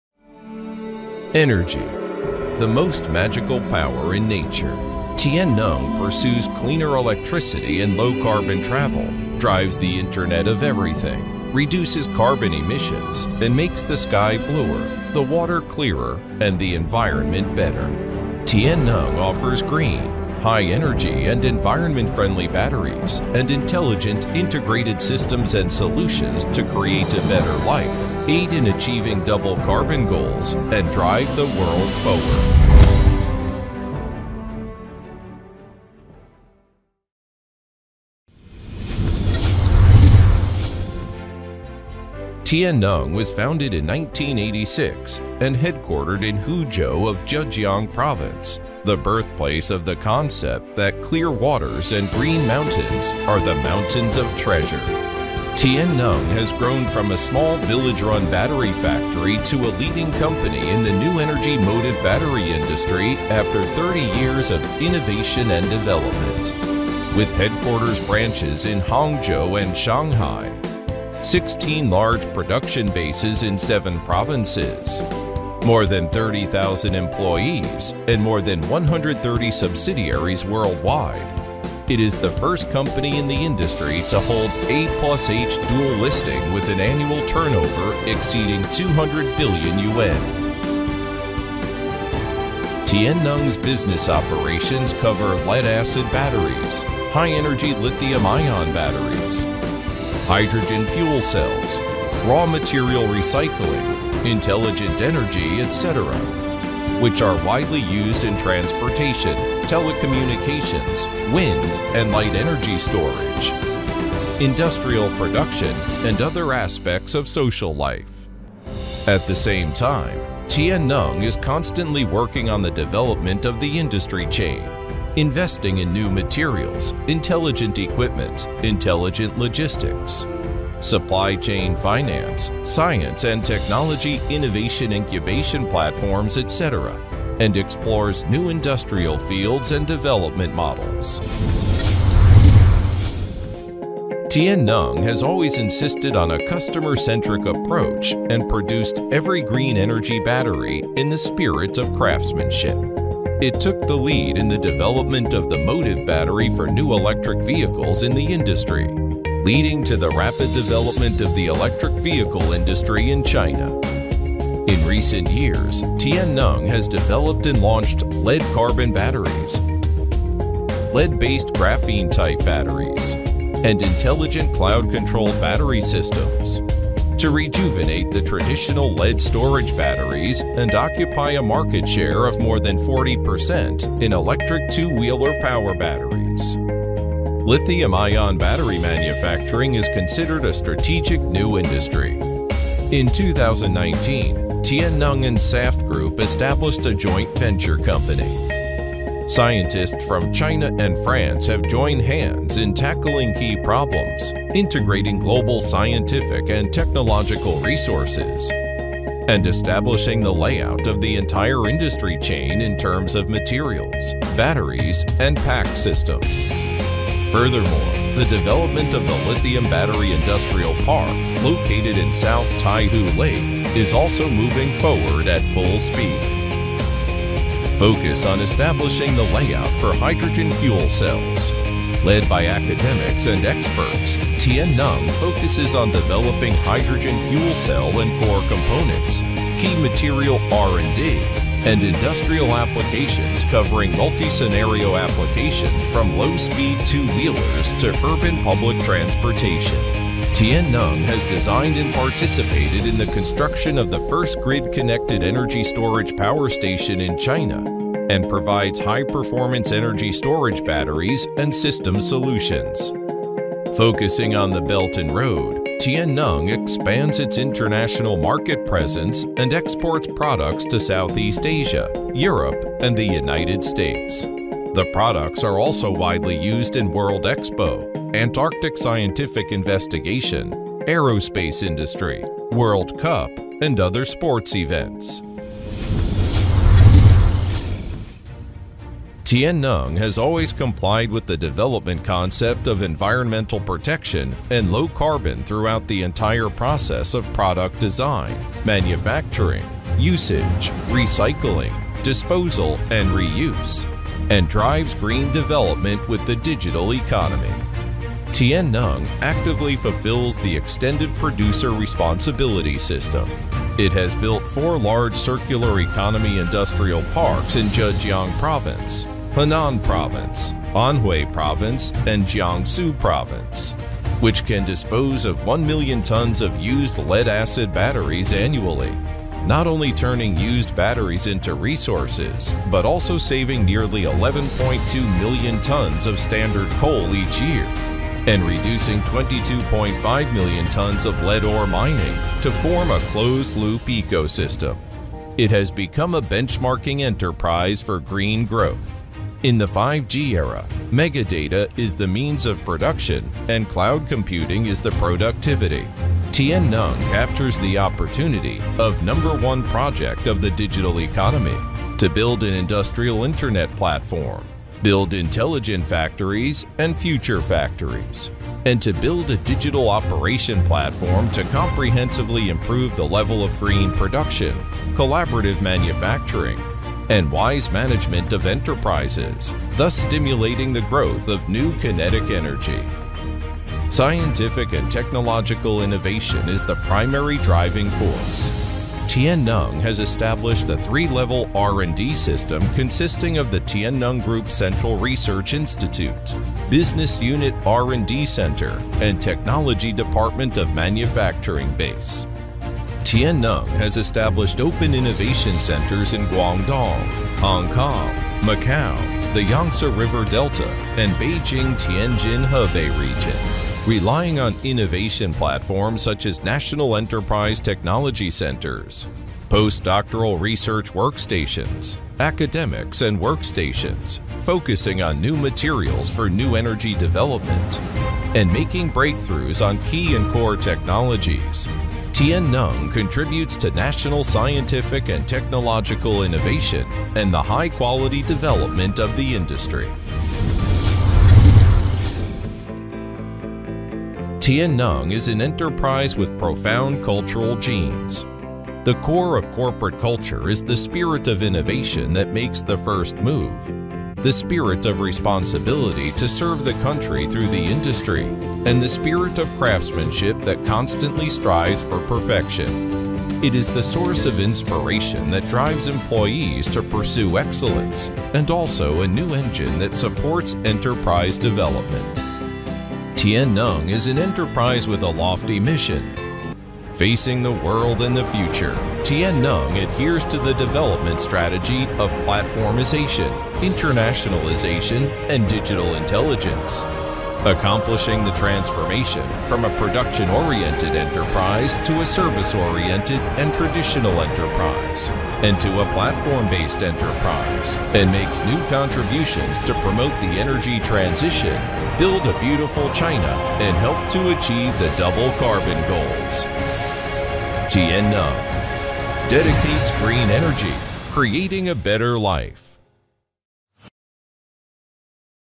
Tianneng Group introduction -Tianneng Battery Company profile
Tianneng Holding Group is a large-scale industrial company dedicated to manufacturing environmentally friendly motive battery for electric vehicle, integrating R&D, production, and sales of new energy vehicle lithium batteries, car stop-start batteries, wind and solar energy storage battery, as well as construction of urban intelligent micro-grid construction and green intellectual industrial park as a whole.The company has built 16 production bases and has more than 100 subsidiaries. It is a leading enterprise of new energy power battery in China.